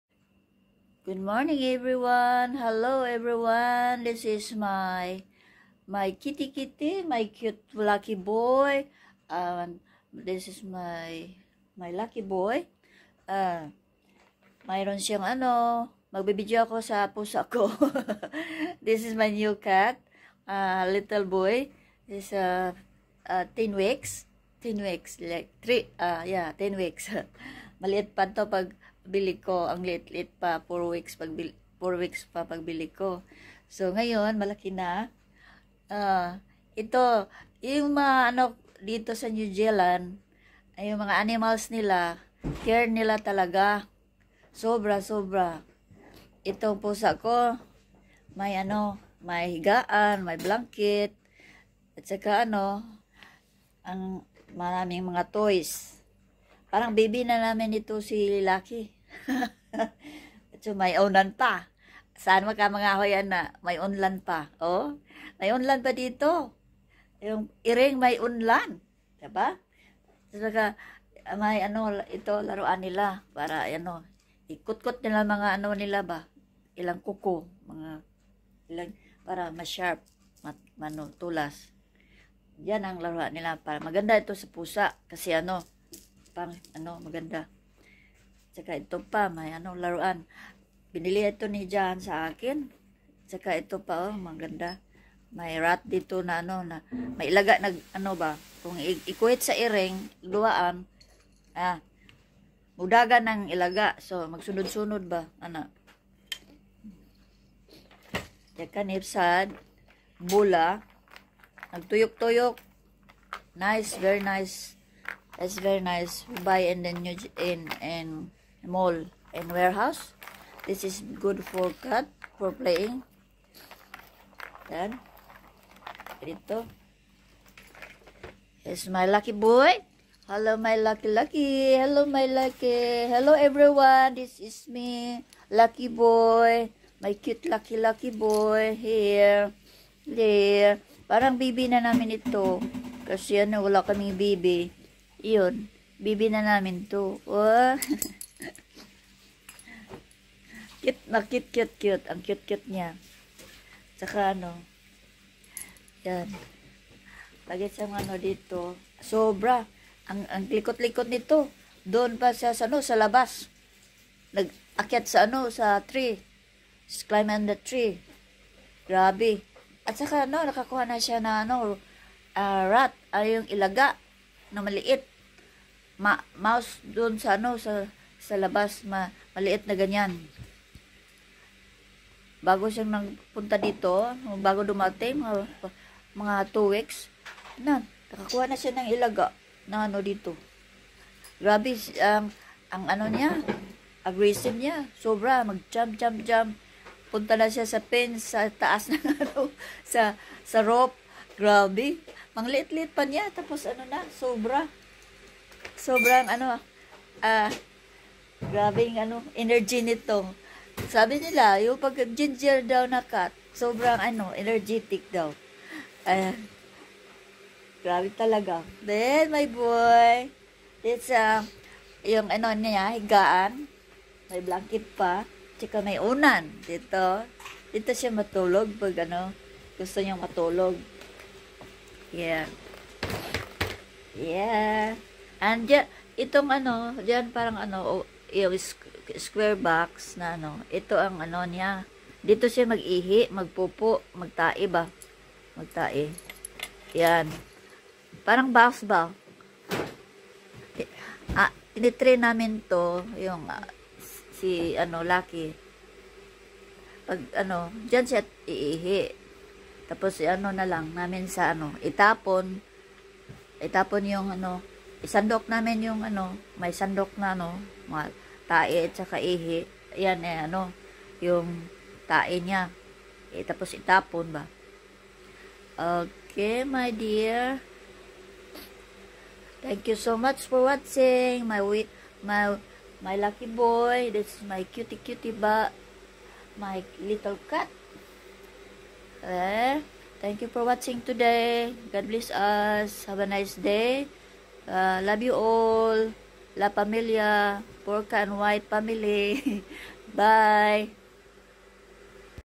Lucky my Cat
This is a short video about my new cat in Christchurch New Zealand